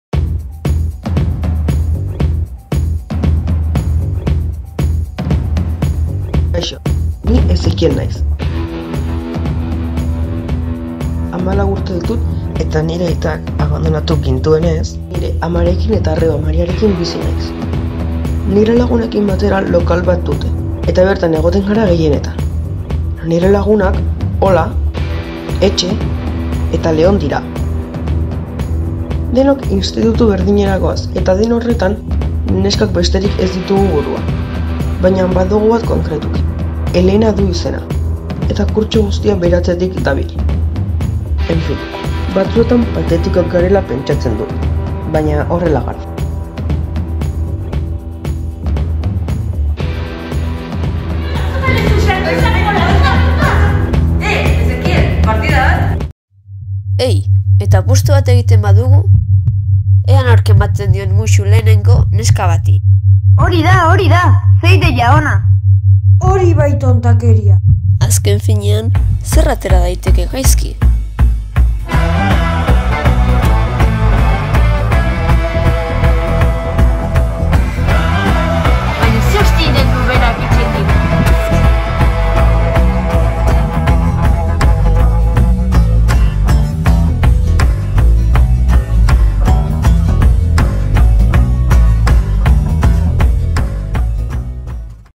2020-21 BookTrailer irabazlea DBH1-2 - Kalekoak talea (Axular Lizeoa)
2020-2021 BookTrailer lehiaketako DBH1-2 kategoriako irabazlea izan da Axular Lizeoko KALEKOAK taldea.

Jasone Osororen Ezekiel liburuari buruzko booktrailerra egin dute.